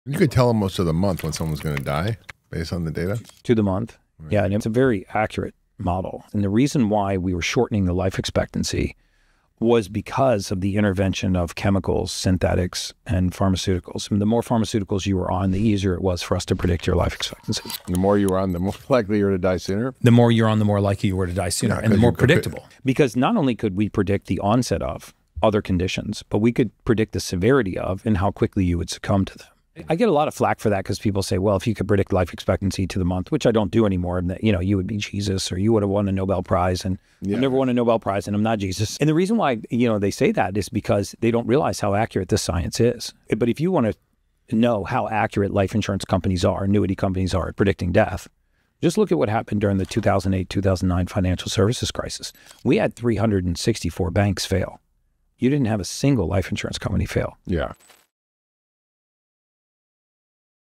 Life Insurance Can Predict Death
Life insurance companies know exactly when you’re going to die. Down to the month.

It sounds like science fiction, but it’s just data. They’ve spent decades collecting data on blood biomarkers, nutrient levels, and lifestyle factors that predict mortality with stunning accuracy. Their risk models are so precise that while 364 banks collapsed in the 2008 financial crisis, not a single life insurance company failed.